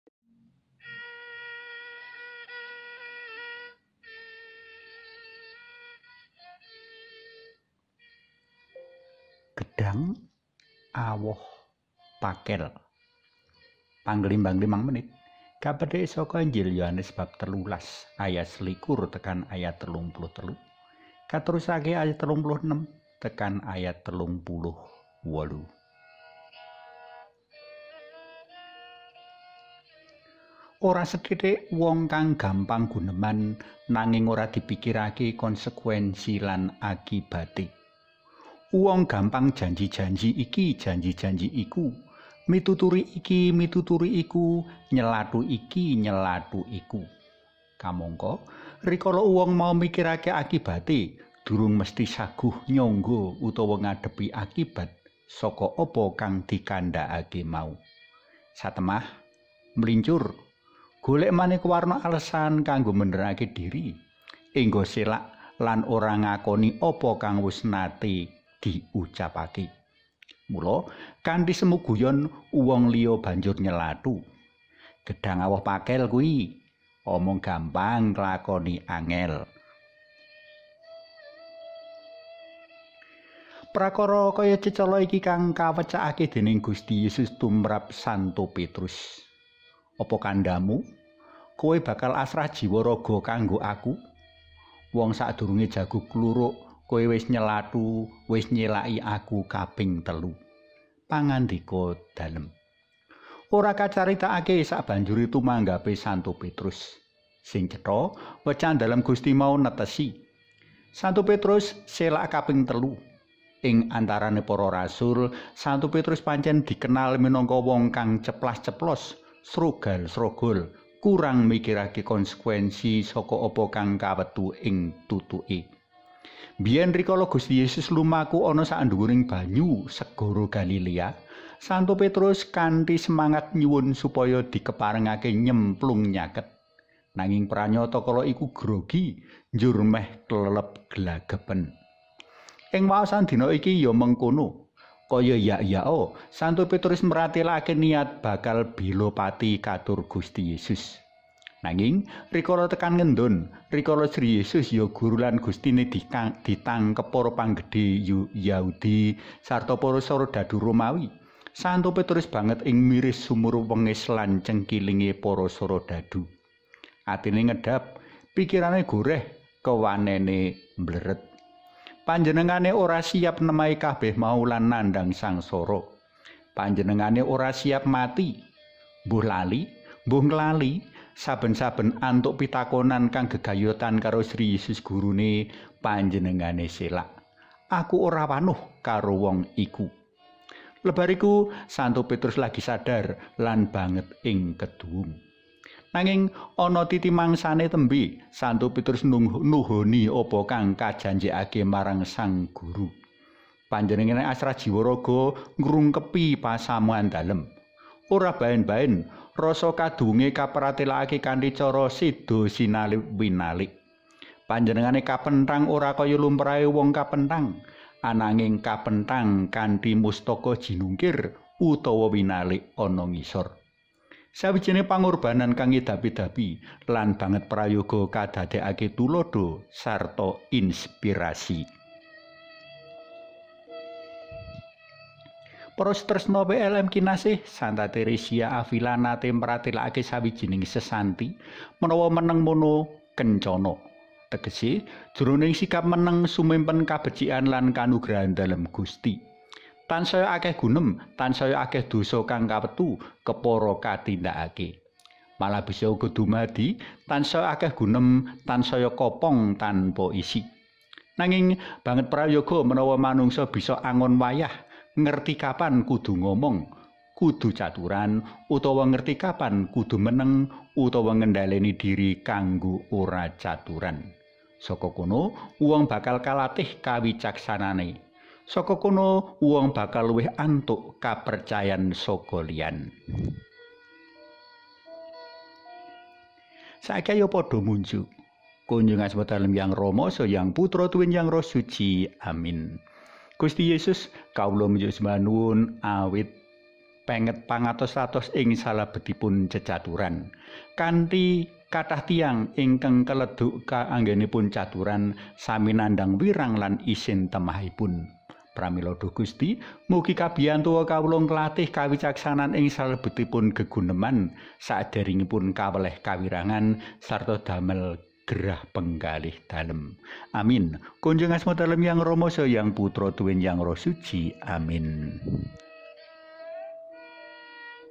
Panglimbang Limang Menit | 4 April 2023 (Selasa-Pekan Suci)| Br. Antonius Mungsi, O. Carm
Panglimbang Limang Menit (Renungan Harian berbahasa Jawa) edisi 4 April 2023 | "Gedhang..." (Yoh. 13:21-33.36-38)
Renungan berbahasa Jawa ini dimaksudkan untuk melestarikan budaya Jawa sembari memelihara iman berdasarkan bacaan-bacaan liturgis Gereja Katolik.